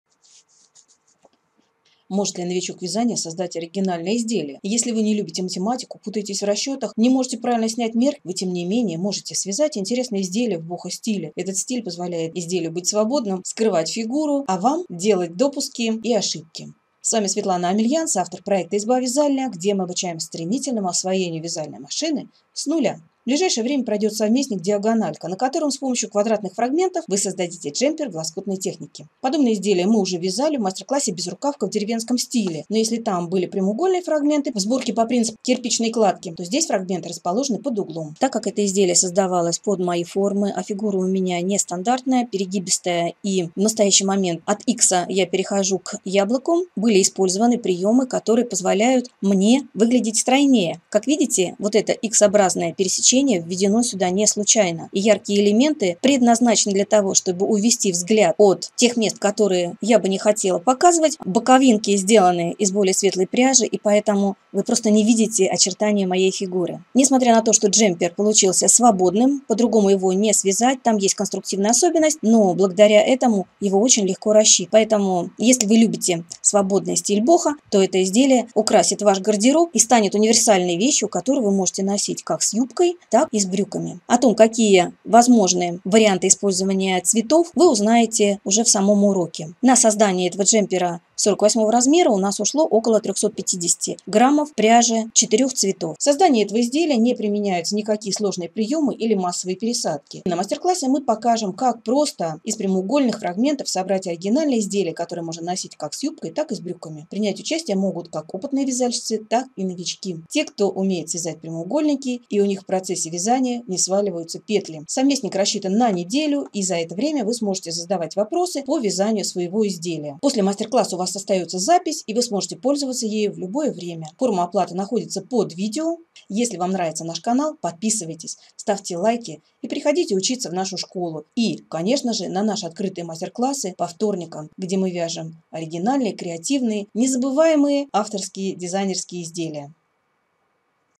Как связать джемпер для дам с пышными формами?
В ближайшее время пройдет совместник “Диагональка”, на котором с помощью квадратных фрагментов вы создадите женский универсальный джемпер в лоскутной технике.
Который идеально подходит к юбке и к брюкам.
 Подобное изделие мы вязали в мк “Безрукавка в деревенском стиле". Но если там были прямоугольные фрагменты в сборке по принципу кирпичной кладки, то здесь фрагменты  расположены  под углом
В  создании этого изделия не применяются никакие сложные приемы или массовые пересадки.
На МК мы покажем как просто из прямоугольных фрагментов собрать оригинальное изделие, которое можно носить как с юбкой, так и с брюками.
Принять участие могут как опытные вязальщицы, так и новички.
Те. кто умеет вязать прямоугольники и у них в  процессе вязания не сваливаются петли.
Совместник рассчитан на неделю и в это время вы можете задавать вопросы по вязанию  своего изделия.

У вас останется запись и вы сможете пользоваться ею в любое время.
 Форма оплаты под видео.

ИЗБА-ВЯЗАЛЬНЯ.рф   kvadrat-n.ru